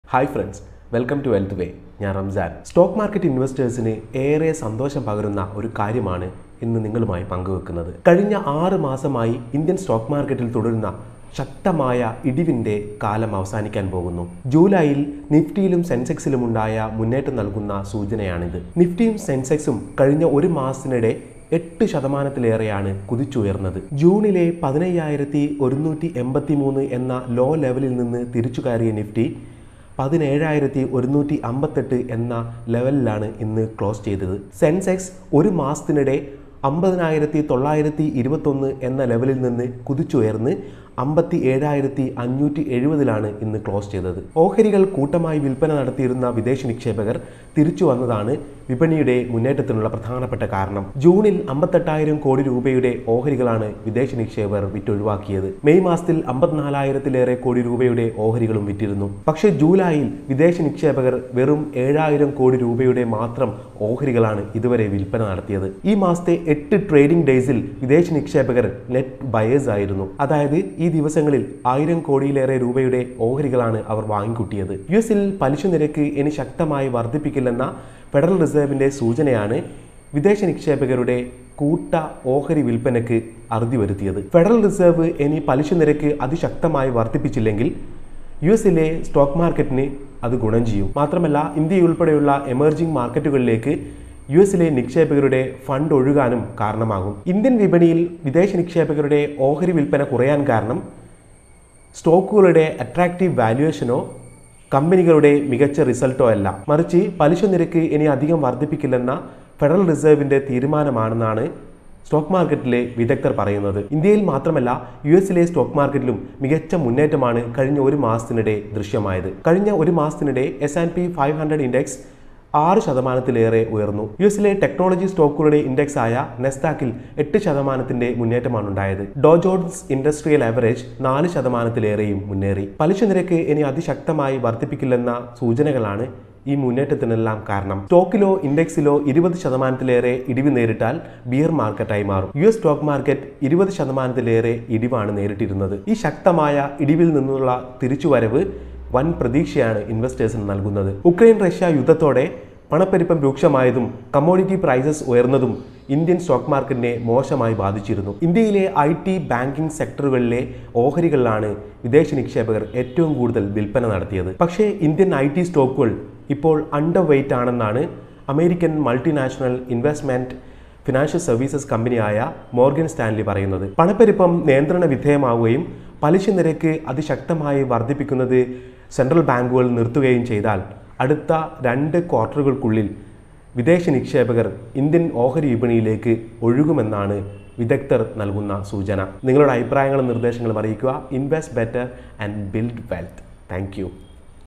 Stock Market പറക്കാൻ ഒരുങ്ങുകയാണോ? വിദേശ നിക്ഷേപകർ നൽകുന്ന സൂചനകൾ ഇതാണ് ..!
Nifty and Sensex are up more than 8%. FIIs reduced sell off in Indian market...What is happening with the market?